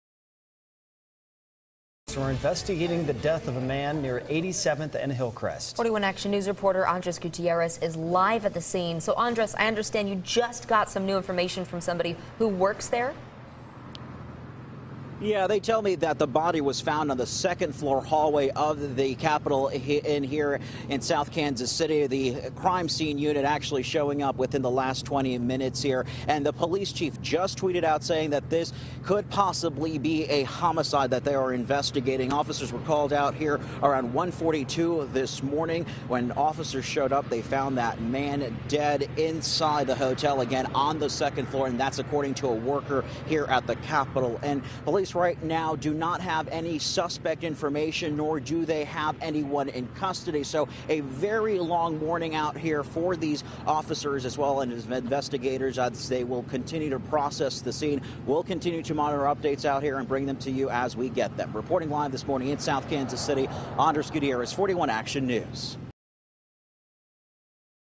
KC police conduct death investigation
Officers are conducting a death investigation at the Capital Inn.

41 Action News, KSHB, brings you the latest news, weather and investigative reports from both sides of the state line.